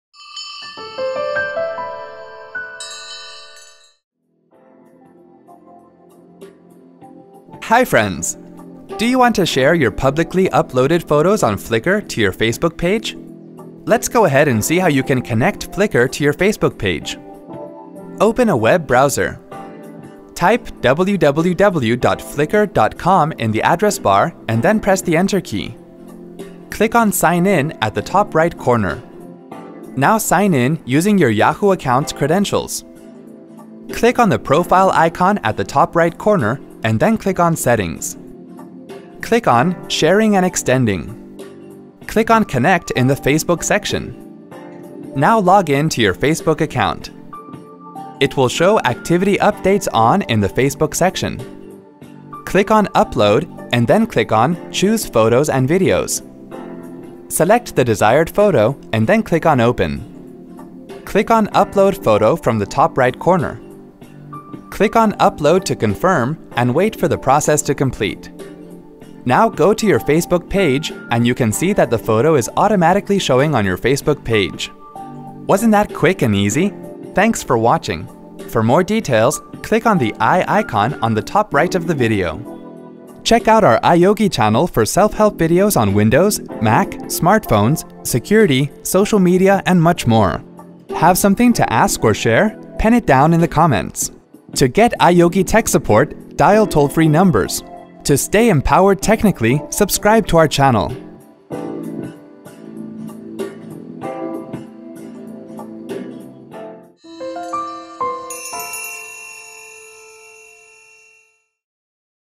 How to connect Flickr® to your Facebook® page :Tutorial
Do you want to share your publicly uploaded photos on Flickr® to your Facebook® page? Get to know about easy steps to connect Flickr® to your Facebook® page by watching this video.